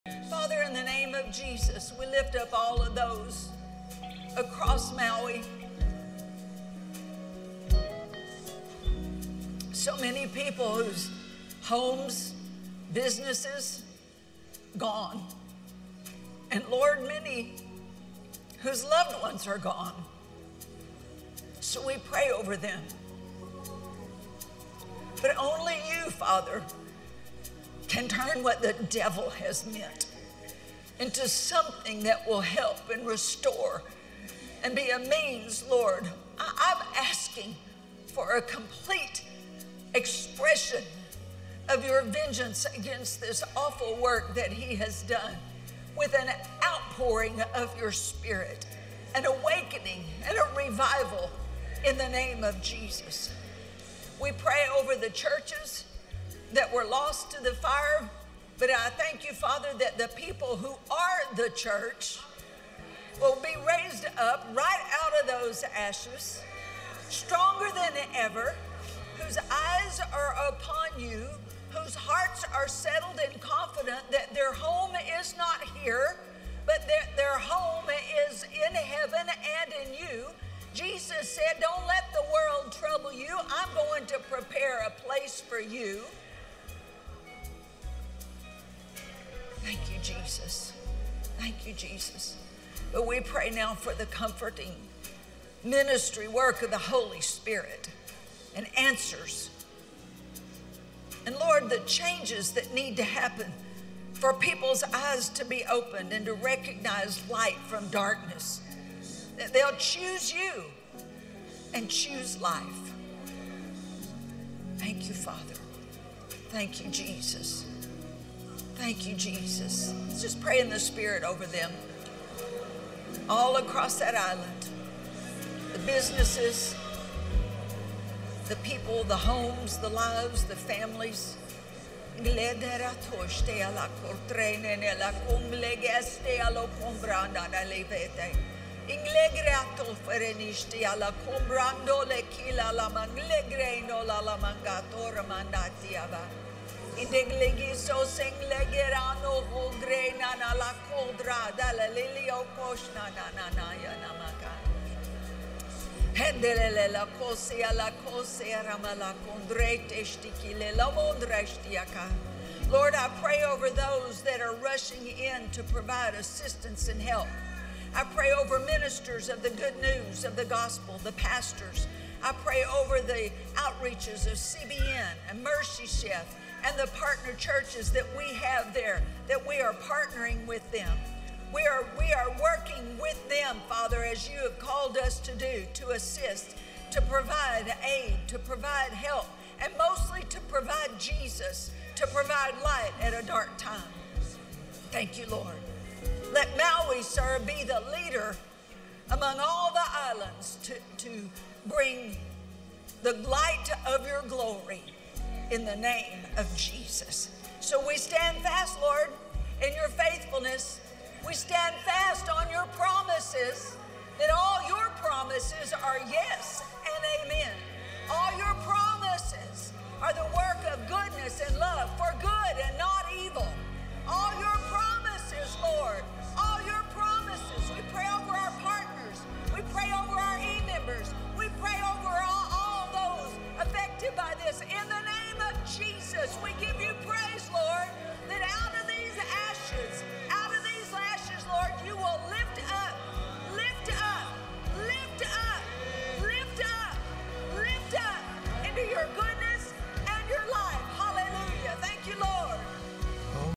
Prayer for Maui
We pray for those who have been impacted in Maui by the wildfires. We pray for restoration to take place and we pray that You, Lord, turnaround what was meant for evil for your good.

We call for complete restoration in Jesus Name!